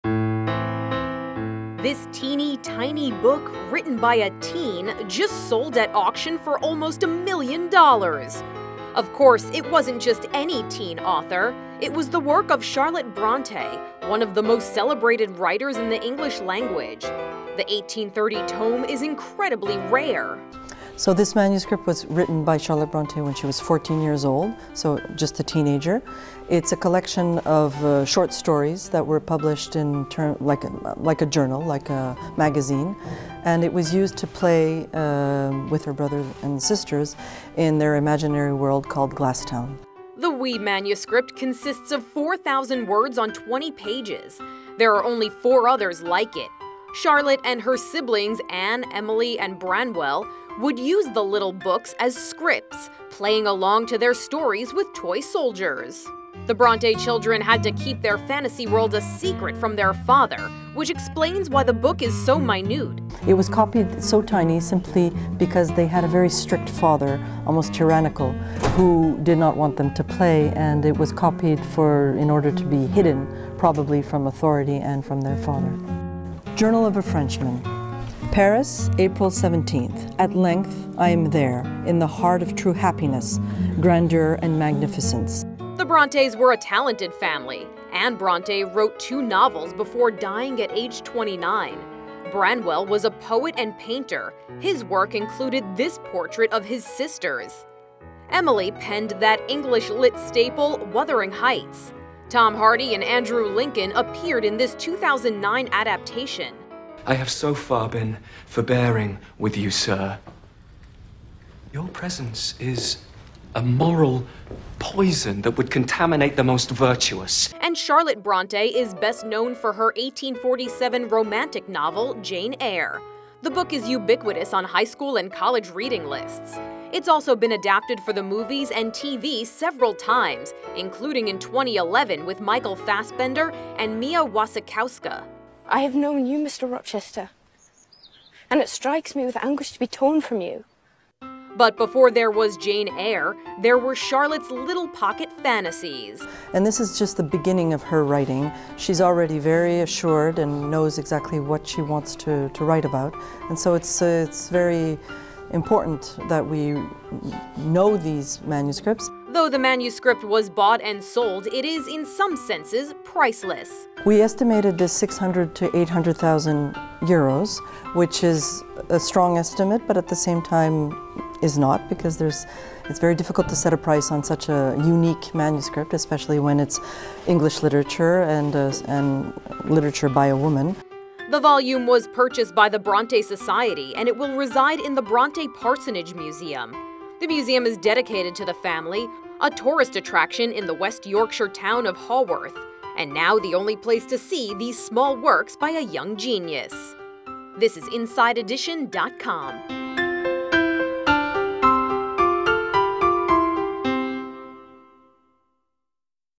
Why This Tiny Book Sold for Almost $1 Million Dollars
A tiny book, written by Charlotte Bronte when she was a teenager, just sold at auction for almost $1 million. The 1830 tome is incredibly rare. Rare books expert Ariane Adeline says, “It's a collection of short stories that were published like a journal, like a magazine. And it was used to play with her siblings - her brother and sisters - in their imaginary world called Glass Town." The wee manuscript consists of 4,000 words on 20 pages. There are only four others like it.